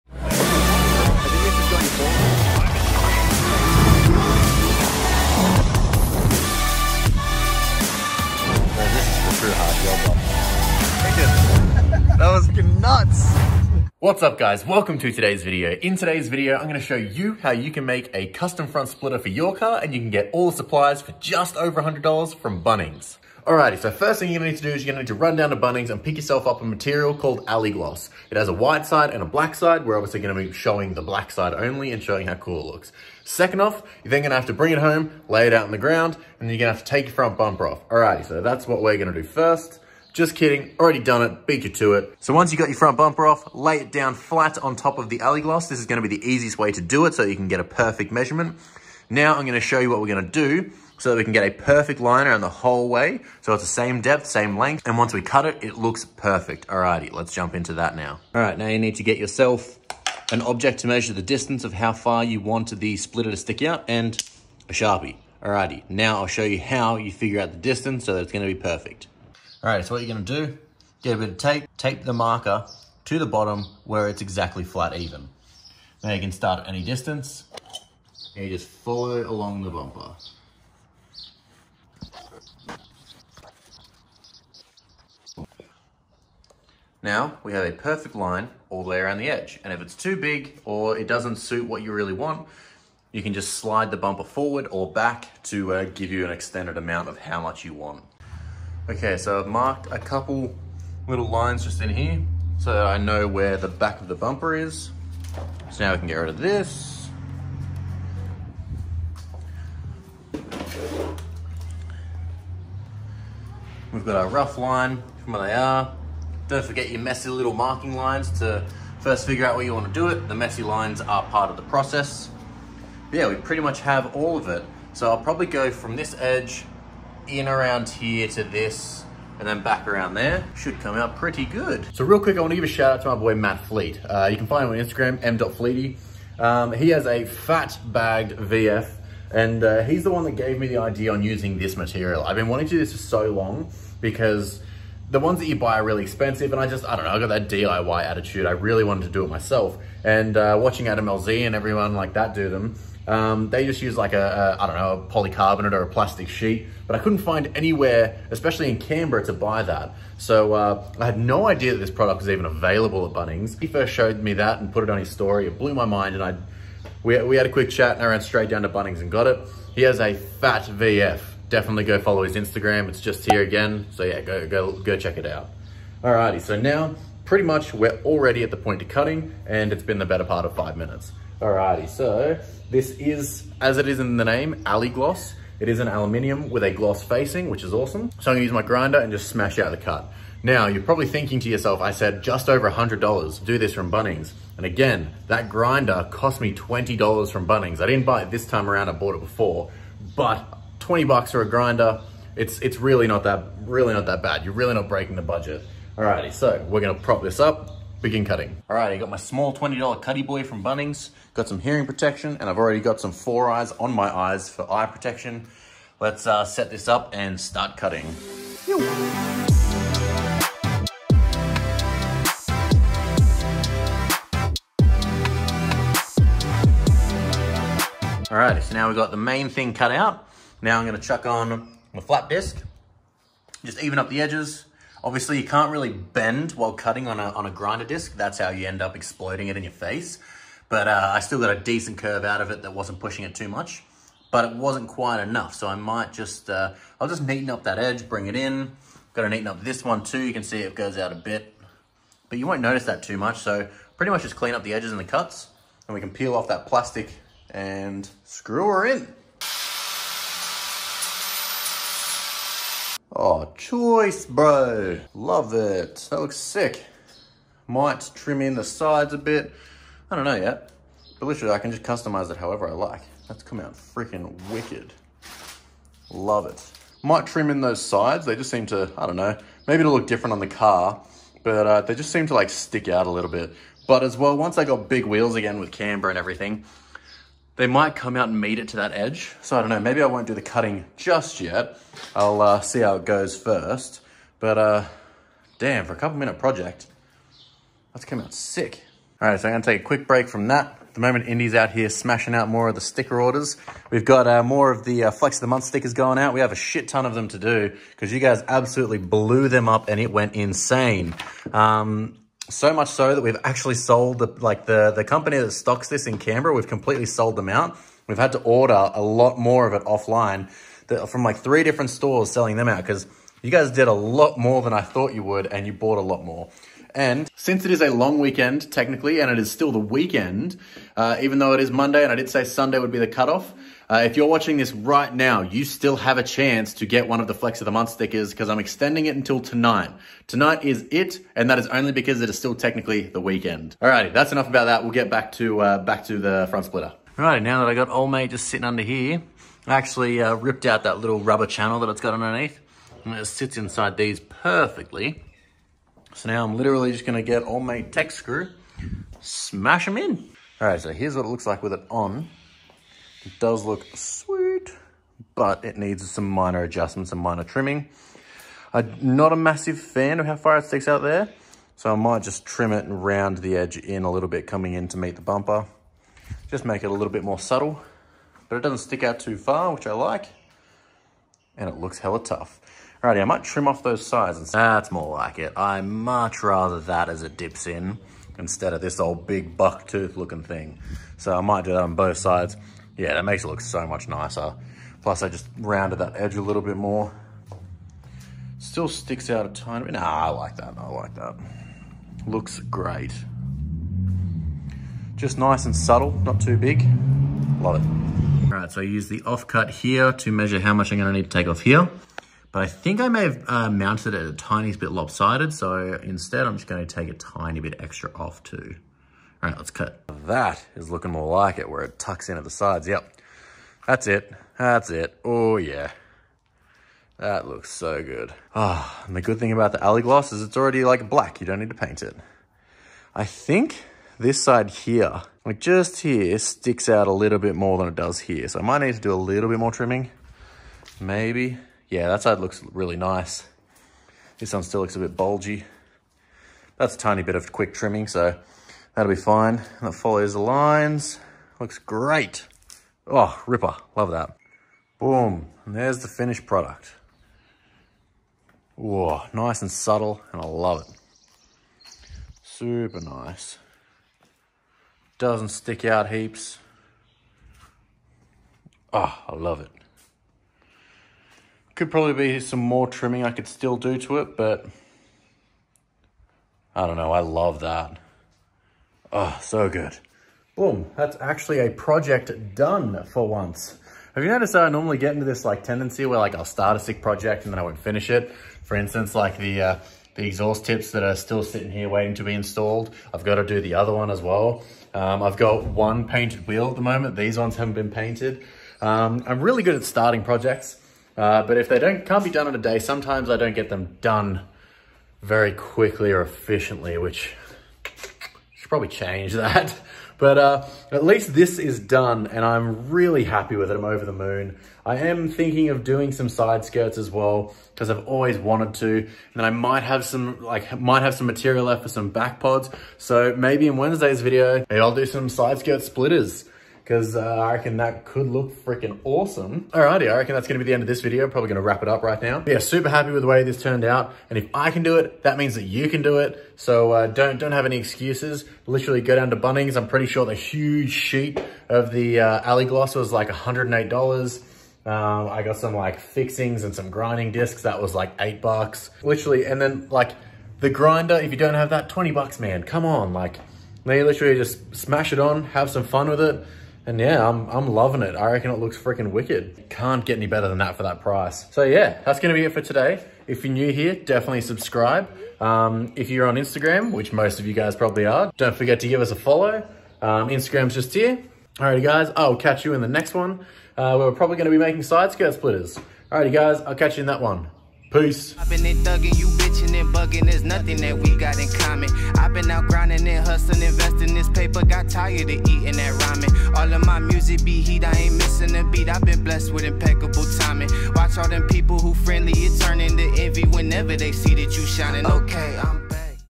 Custom Front Splitter From Bunnings For Less Than $120 - Tutorial
Make your own DIY custom front splitter for suuuuper cheap using materials only found at your local bunnings!

Picnik Stickers